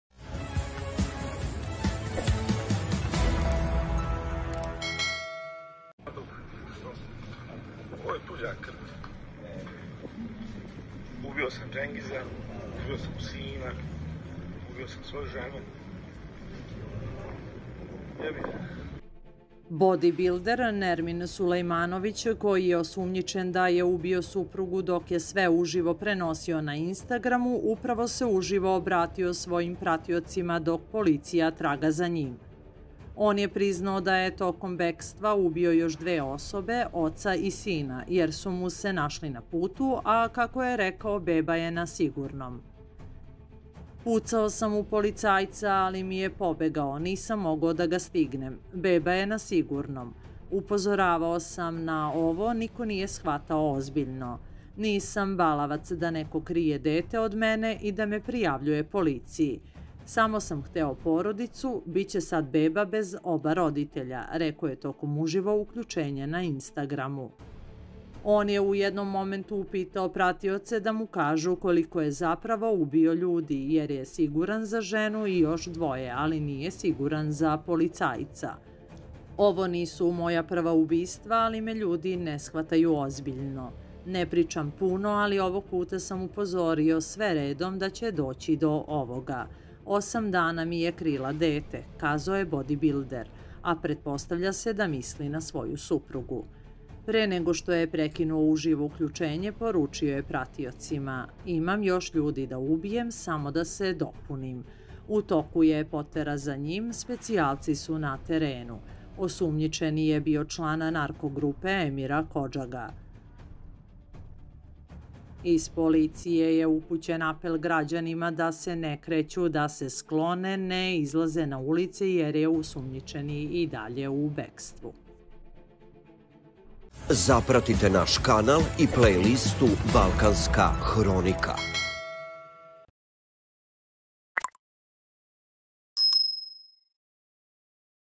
HAOS U BIH! UBICA U BEKSTVU! POZNATI BODIBILDER UBIO ŽENU I JOŠ DVOJE! NAJAVIO JOŠ!
Label and copyright: Vasilis produkcija